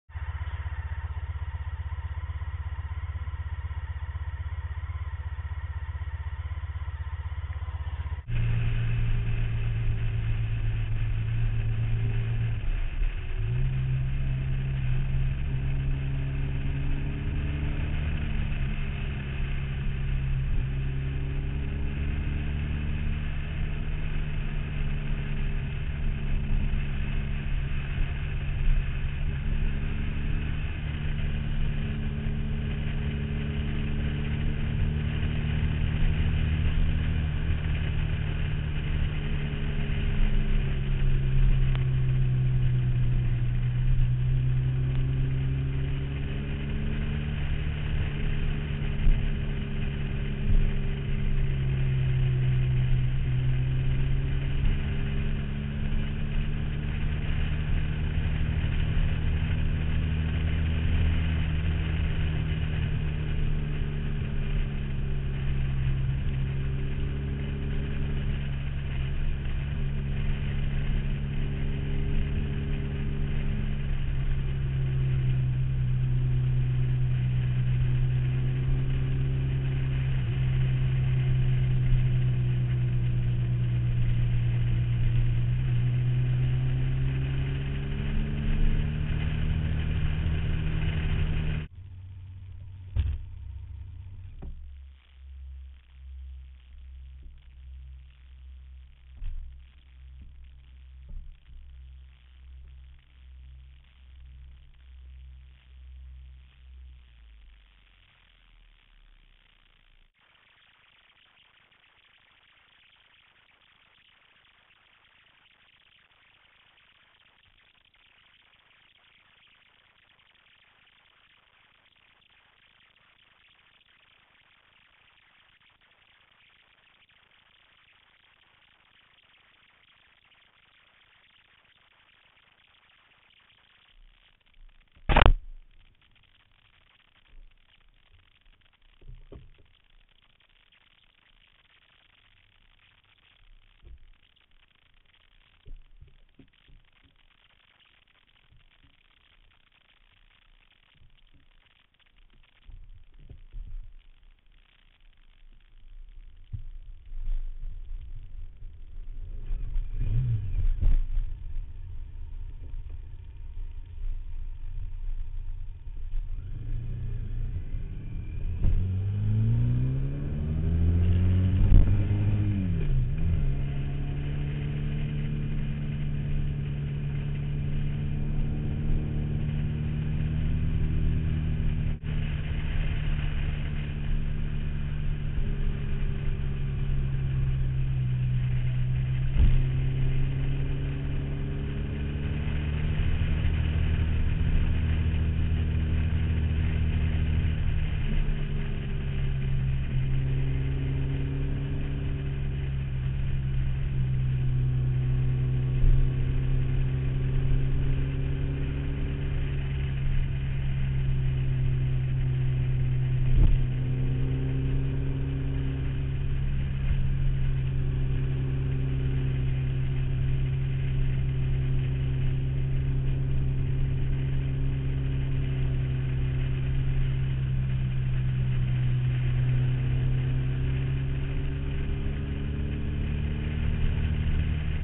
Rockpile Rd., Sonoma County.
Local Road which doesn't show as well as it rides. Nice move at 2:10 into the video. It was just my shadow and I out this day. The camera was mounted on the left side of my saddlebag. It had a high pitched " rustle" to the sound and the exhausts didn't sound as good as when the camera was up on the mirror.